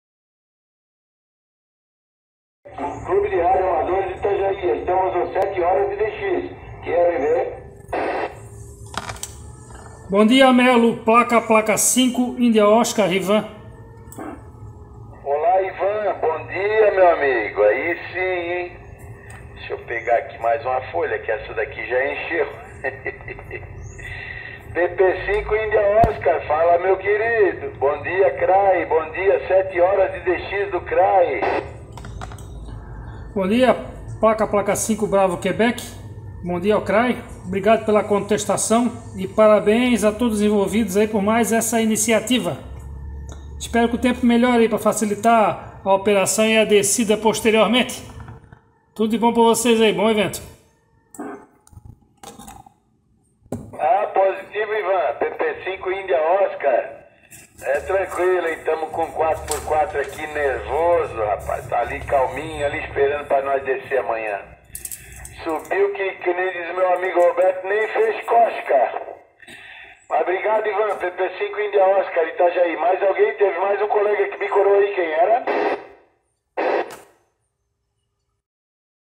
Clube de Radioamadores Itajaí QTH morro do Brilhante PP5DQ evento VHF 40 anos PP5IO PP5BQ (QTC)
A SUA PARTICIPAÇÃO É MUITO IMPORTANTE!

Participação no evento alusivo a  comemoração dos 40 anos do CRAI-Clube de Radioamadores de Itajaí- SC  PP5DQ  operando em VHF/FM 146.400 / Simplex,  no dia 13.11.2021  das 07:00 ás 14:00 .
Grid Locator - GG520W

Registro do momento marcando presença e parabéns a todos de PP5IO Ivan. 73!